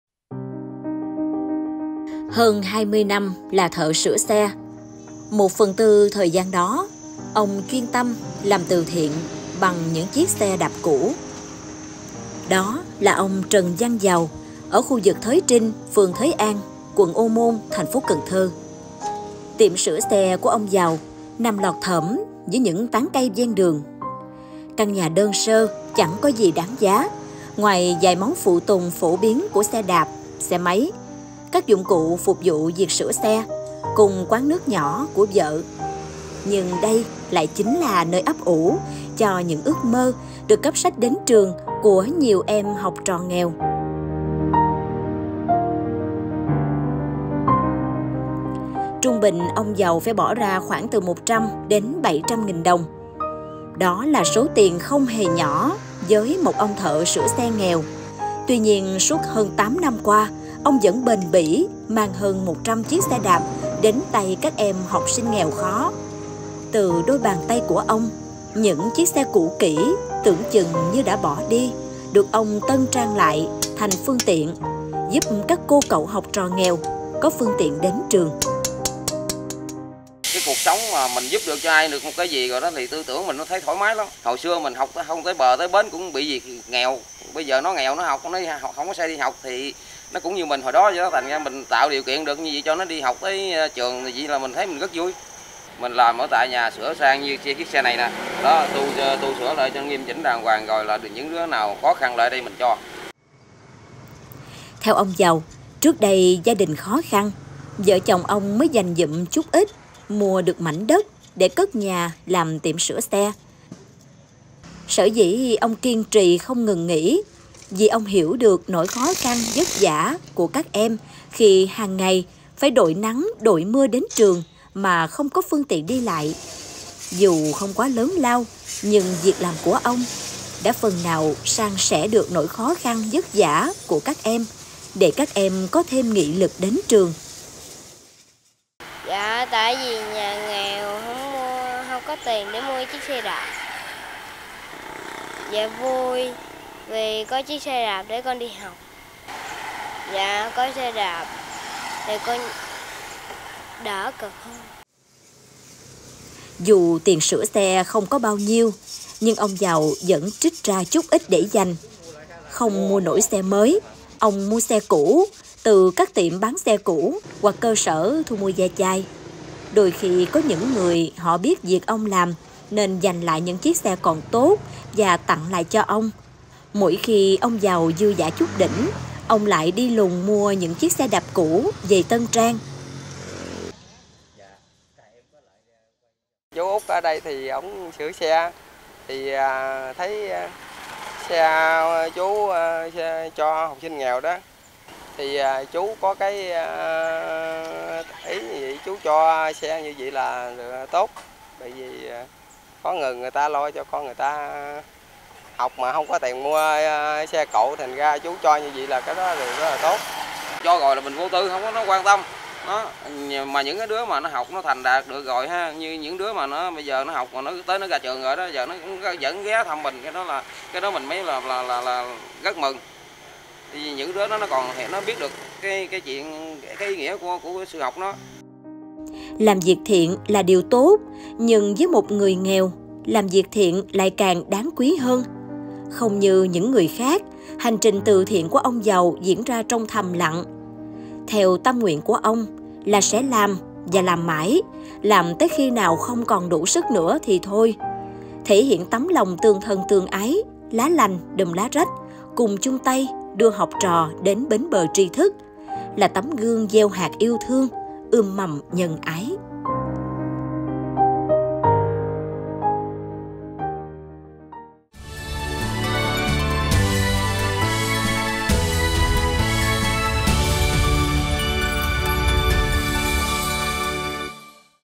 Những chiếc xe đạp mang nặng nghĩa tình | Người tốt việc tốt
Hơn 20 năm là thợ sửa xe, 1/4 thời gian đó ông chuyên tâm làm từ thiện bằng những chiếc xe đạp cũ. Đó là ông Trần Văn Giàu ở khu vực Thới Trinh, phường Thới An, quận Ô Môn, thành phố Cần Thơ. Tiệm sửa xe của ông Giàu nằm lọt thỏm giữa những tán cây ven đường. Căn nhà đơn sơ chẳng có gì đáng giá, ngoài vài món phụ tùng phổ biến của xe đạp, xe máy, các dụng cụ phục vụ việc sửa xe, cùng quán nước nhỏ của vợ. Nhưng đây lại chính là nơi ấp ủ cho những ước mơ được cắp sách đến trường của nhiều em học trò nghèo.